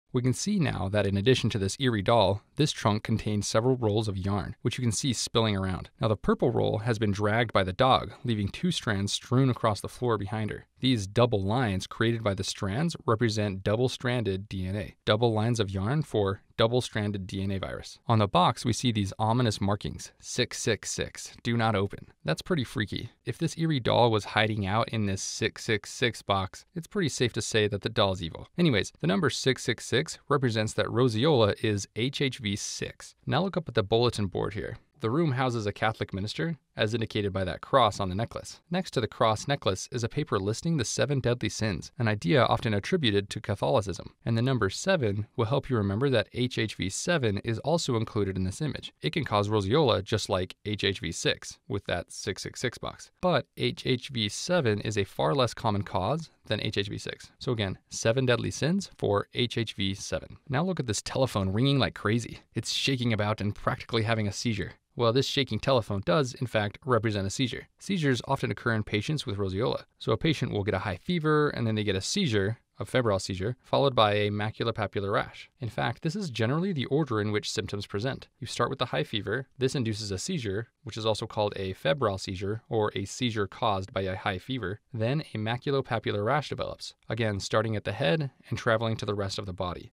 Roseola — Why this simple childhood virus keeps showing up on exams
For USMLE for beginners, this is a must-know. Understand how HHV-6 causes that high fever before the rash — and why recognizing the order matters for Step 1 and Step 2.